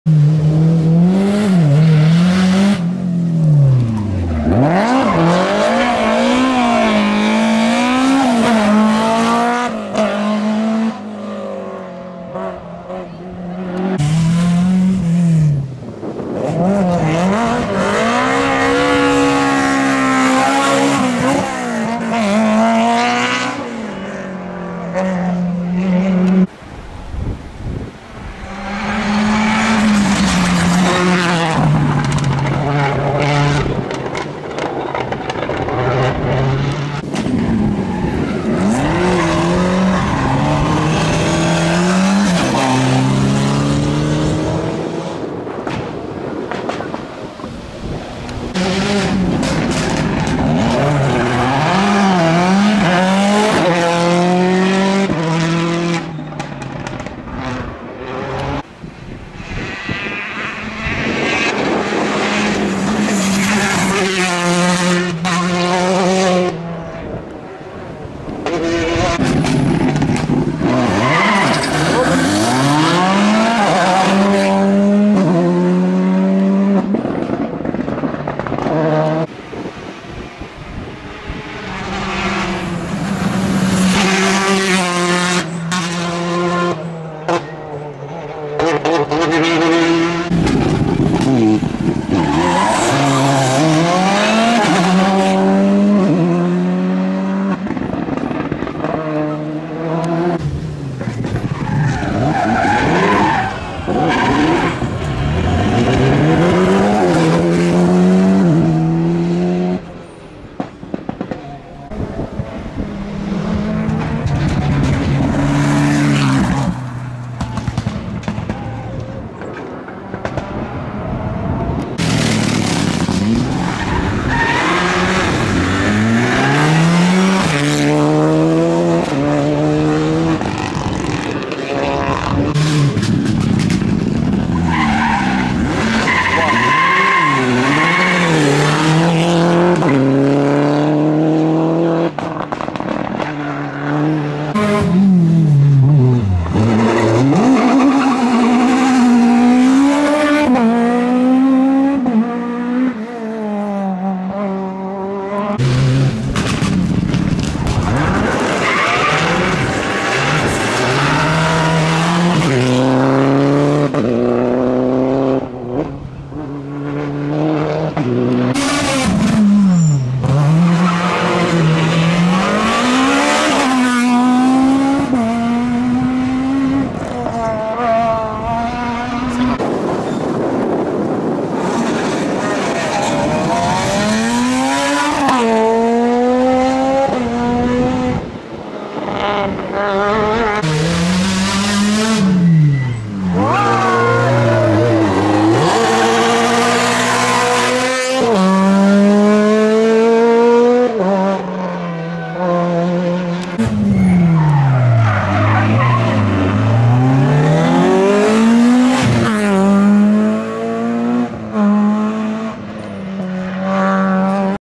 Rallye du Bassin Annonéen 2024
Je n'étais pas présent sur l'épreuve donc cette fois-ci je possède un nombre de passage restreint.
Merci à Bastien et Tom pour leurs images